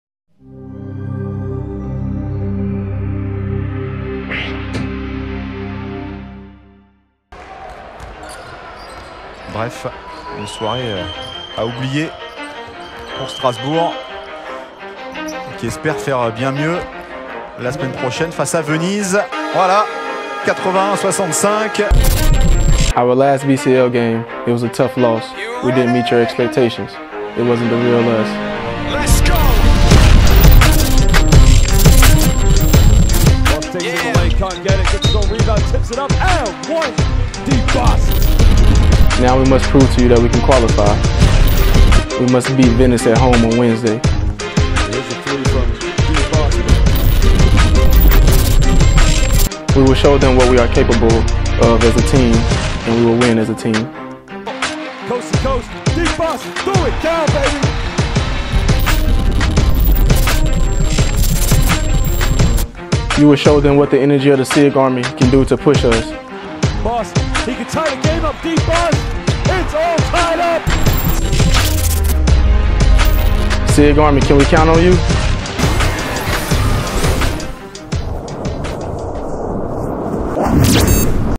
Teaser SIG Strasbourg / Venise
La SIG Strasbourg est en quête de rachat au moment d’accueillir Venise en Basketball Champions League. Dee Bost en a conscience et a un message pour la SIGArmy afin de montrer à tous que l'envie de se qualifier est bien présente ...